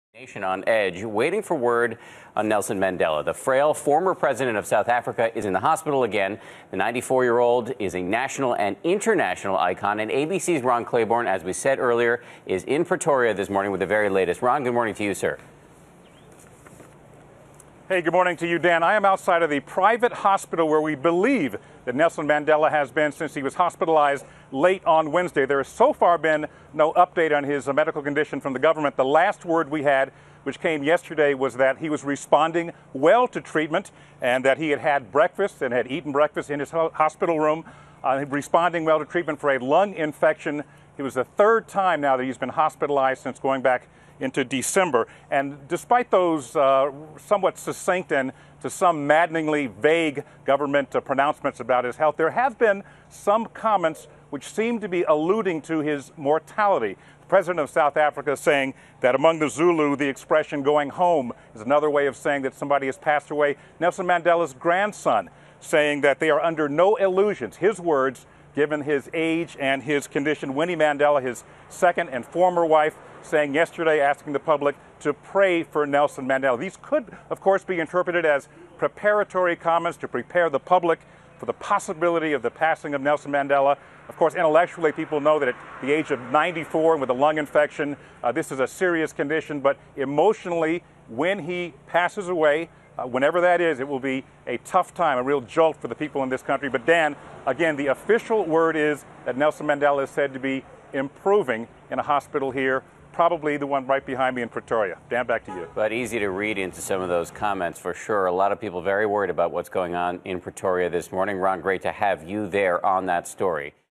Nelson Mandela: World Awaits the Outcome of his Hospital Visi
An update of former president of South Africa, Nobel Peace Prize winner's health.